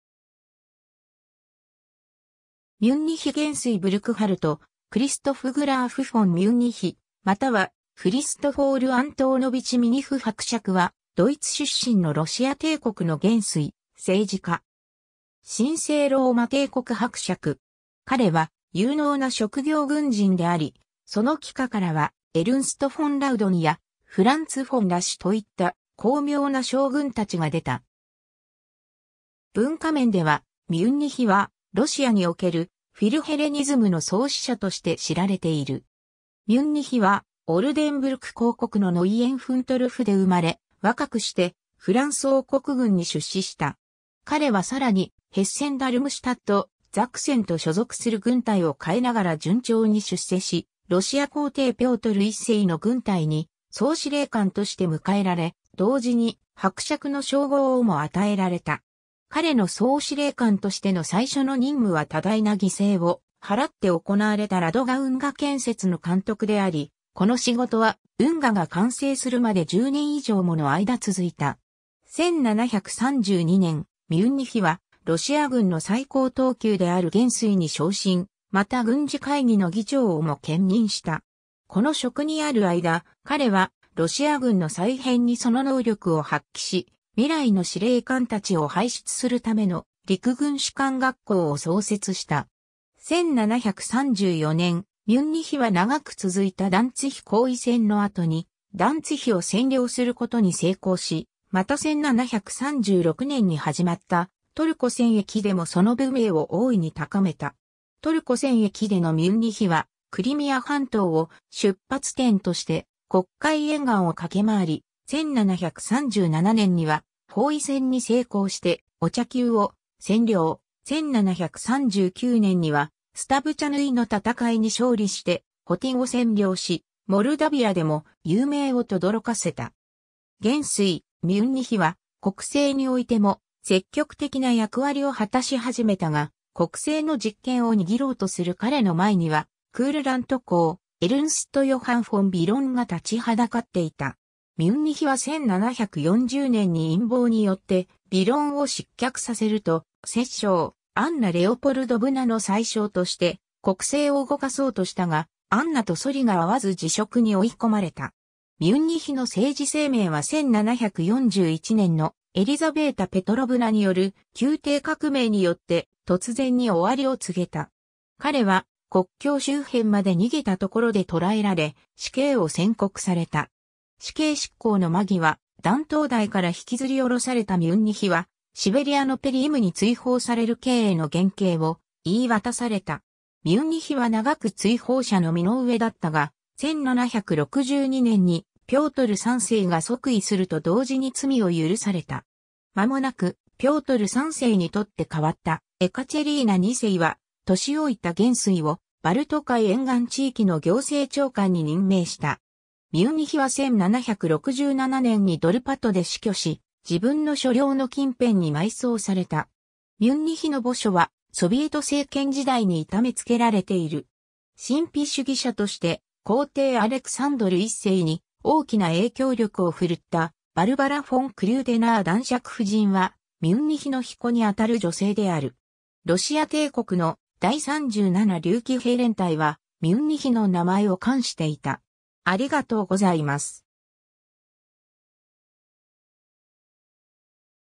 ブルクハルト・クリストフ・フォン・ミュンニヒ
アンナ_(ロシア皇帝)
オルデンブルク_(領邦)
ミュンニヒ元帥 ブルクハルト・クリストフ・グラーフ・フォン・ミュンニヒ（ドイツ語: Burkhard Christoph Graf von Münnich、1683年5月9日 - 1767年10月16日）またはフリストフォール・アントーノヴィチ・ミニフ伯爵（ロシア語: Христофо́р Анто́нович Миних）は、ドイツ出身のロシア帝国の元帥、政治家。
神聖ローマ帝国伯爵。
彼は有能な職業軍人であり、その麾下からはエルンスト・フォン・ラウドンやフランツ・フォン・ラシといった高名な将軍たちが出た。
文化面では、ミュンニヒはロシアにおけるフィルヘレニズム（ギリシア愛好趣味）の創始者として知られている。
 ミュンニヒはオルデンブルク公国のノイエンフントルフで生れ、若くしてフランス王国軍に出仕した。
彼はさらにヘッセン＝ダルムシュタット、ザクセンと所属する軍隊を変えながら順調に出世し、ロシア皇帝ピョートル1世の軍隊に総司令官（ロシア軍の序列では第2位）として迎えられ、同時に伯爵の称号をも与えられた。
彼の総司令官としての最初の任務は多大な犠牲を払って行われたラドガ運河建設の監督であり、この仕事は運河が完成するまで10年以上もの間続いた。
 1732年、ミュンニヒはロシア軍の最高等級である元帥に昇進、また軍事会議の議長をも兼任した。
この職にある間、彼はロシア軍の再編にその能力を発揮し、未来の司令官たちを輩出するための陸軍士官学校を創設した。
1734年、ミュンニヒは長く続いたダンツィヒ攻囲戦の後にダンツィヒを占領することに成功し、また1736年に始まったトルコ戦役でもその武名を大いに高めた。
トルコ戦役でのミュンニヒはクリミア半島を出発点として黒海沿岸を駆け回り、1737年には包囲戦に成功してオチャキウを占領、1739年にはスタヴチャヌィの戦いに勝利してホティンを占領し、モルダヴィアでも勇名を轟かせた。
元帥ミュンニヒは国政においても積極的な役割を果たし始めたが、国政の実権を握ろうとする彼の前にはクールラント公エルンスト・ヨハン・フォン・ビロンが立ちはだかっていた。
 ミュンニヒは1740年に陰謀によってビロンを失脚させると、摂政アンナ・レオポルドヴナの宰相として国政を動かそうとしたが、アンナとそりが合わず辞職に追い込まれた。
ミュンニヒの政治生命は1741年のエリザヴェータ・ペトロヴナによる宮廷革命によって突然に終わりを告げた。
彼は国境周辺まで逃げたところで捕えられ、死刑を宣告された。
死刑執行の間際、断頭台から引きずりおろされたミュンニヒはシベリアのペリムに追放される刑への減刑を言い渡された。
ミュンニヒは長く追放者の身の上だったが、1762年にピョートル3世が即位すると同時に罪を許された。
まもなくピョートル3世に取って代わったエカチェリーナ2世は、年老いた元帥をバルト海沿岸地域の行政長官に任命した。
 ミュンニヒは1767年にドルパトで死去し、自分の所領の近辺に埋葬された。
ミュンニヒの墓所はソビエト政権時代に痛めつけられている。
神秘主義者として皇帝アレクサンドル1世に大きな影響力をふるったバルバラ・フォン・クリューデナー男爵夫人（クリュデネル夫人）は、ミュンニヒの曾孫にあたる女性である。
ロシア帝国の第37竜騎兵連隊は、ミュンニヒの名前を冠していた。